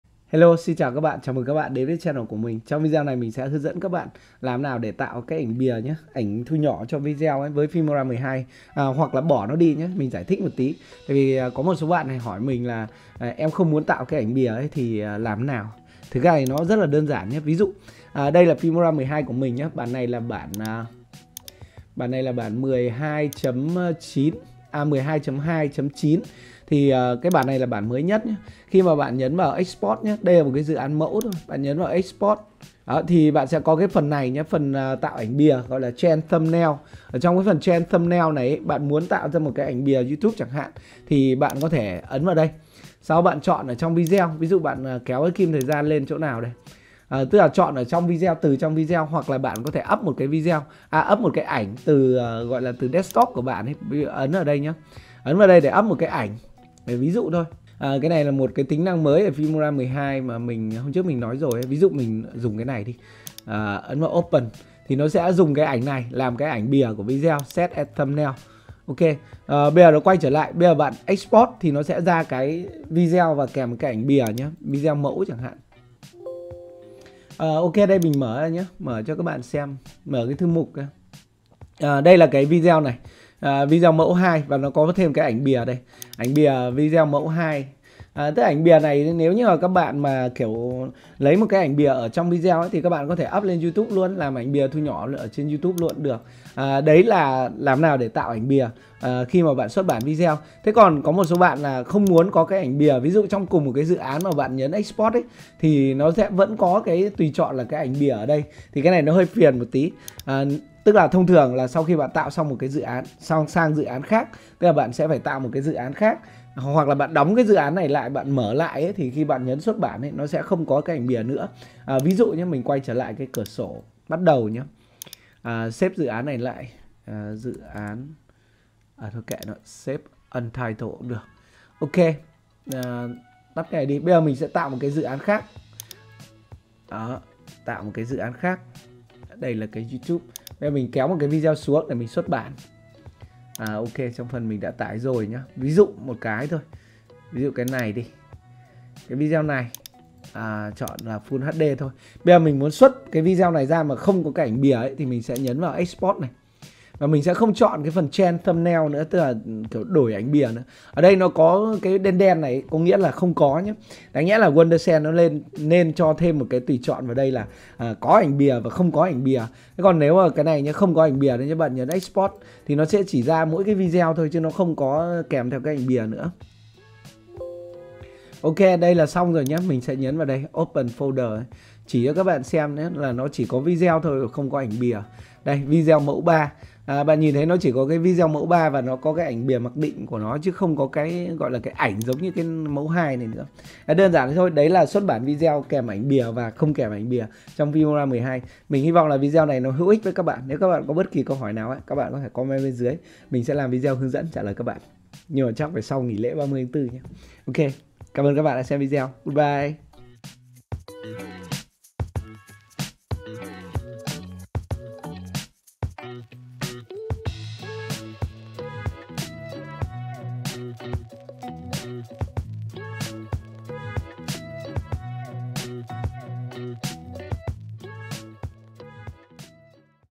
Hướng Dẫn Xuất Bản Video Kèm Ảnh Bìa trong Filmora 12
Tham khảo dịch vụ của Elearning Supporter Tham khảo giá phần mềm Filmora 13 mới nhất ở đây đã nhé Trong video này mình sẽ hướng dẫn các bạn cách xuất bản video kèm ảnh bìa (hoặc không ảnh bìa) với phần mềm Filmora 12.

Anh em nào có tiền đầu tư thì mua nhạc ở trang này nhé: (Audiio)
- Nhạc xin cho filmmakers!